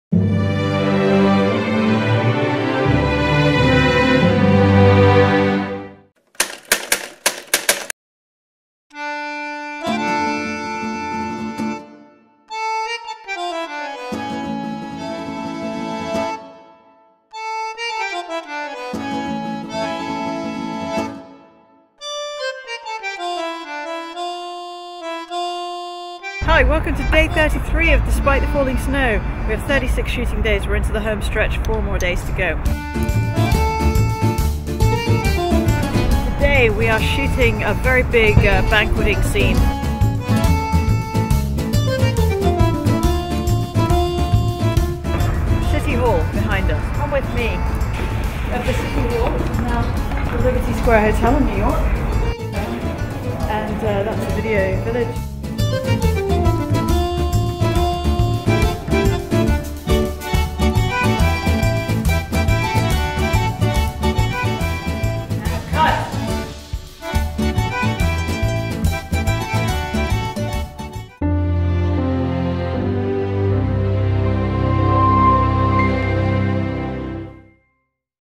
Despite the Falling Snow-Shoot day 33
Director Shamim Sarif and Producer Hanan Kattan filming Despite the Falling Snow; Shoot day 33